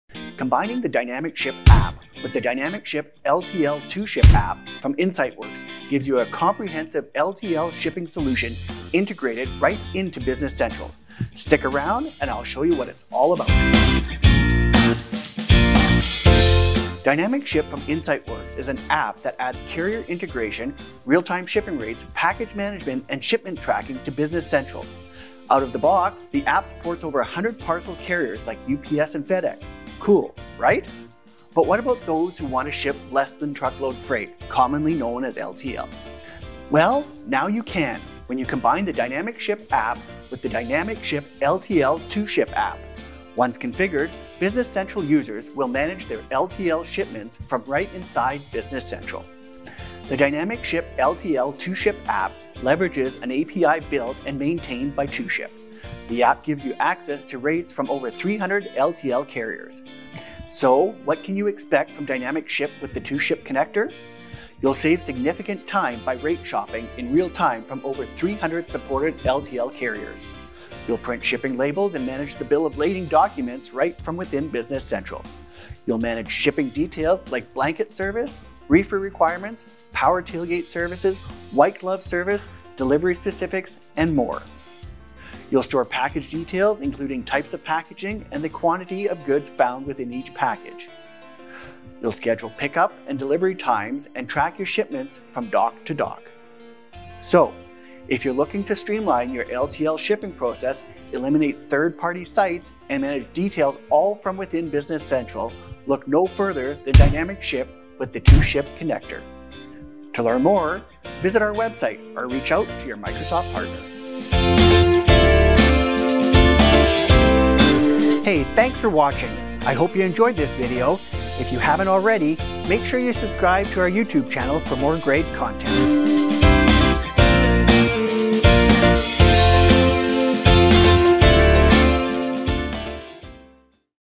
Dynamic Ship LTL 2Ship - Quick Overview
Easily manage your outgoing LTL shipments from Business Central with carrier integration from 2Ship.com, rate shopping, package management, and tracking.

When outbound LTL shipping is vital to your business, there is little room for error, and that’s why companies turn to the Dynamic Ship LTL 2Ship app from Insight Works. Designed specifically for Microsoft Dynamics 365 Business Central, Dynamic Ship LTL 2Ship helps manage your entire outbound LTL shipping process.